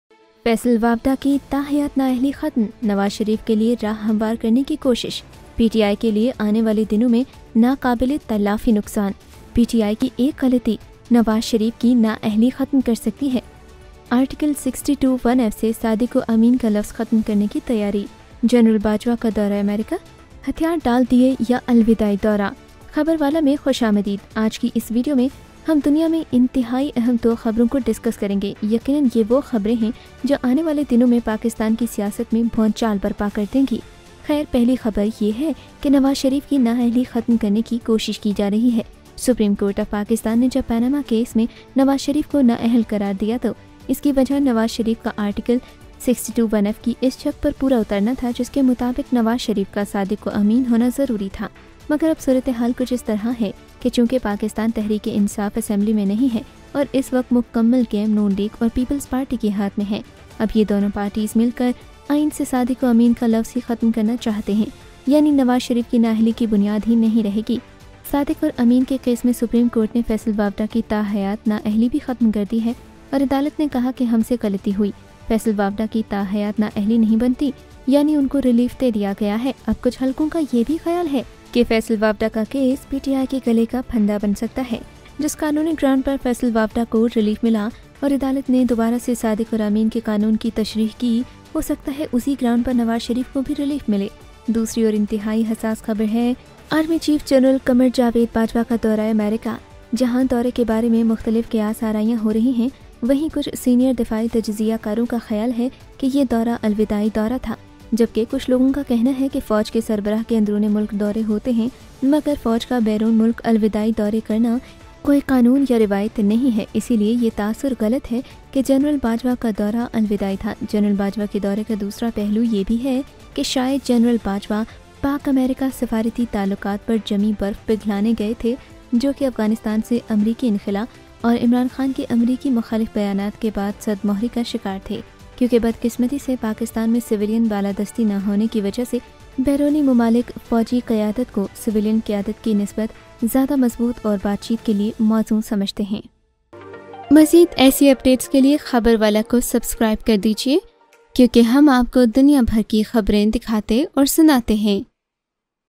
One Mistake Committed By PTI Could End Nawaz Sharif's Disqualification
1. Efforts are being made to remove the disqualification of Nawaz Sharif, when the Supreme Court of Pakistan disqualified Nawaz Sharif in the Panama case, the reason for this was that Nawaz Sharif did not comply with the provisions of Article 62 One F, in case of Nawaz Sharif, it was necessary to be honest and trustworthy. but now the situation is something like this because Pakistan Tehreek-e-Insaf is not in the assembly and at that time the whole game is in the hands of the Non-League and the People's Party. They want to remove only the word Sadiq and Amin, which means that Nawaz Sharif's disqualification will no longer be the basis. Faisal Vawda, who made a mistake on our part, is not disabled for life, that is, he has been given relief.

2. Army Chief General Qamar Javed Bajwa's visit to the United States, where there are various speculations about the visit, some senior defense analysts believe that this visit was a farewell visit, while some people say that the chief of the army inside There are visits to the country, but there is no law or tradition of the army to conduct farewell visits abroad, so this impression is wrong that General Bajwa's visit was a farewell visit. The United States went to melt the ice on diplomatic relations, which were cold after the American withdrawal from Afghanistan and Imran Khan's anti-American statements.

-Bringing you latest stories and news from all over the world.

- hum latay hai aap k lie dunya bhar say taza tareen khabrain or behtareen kahaniyan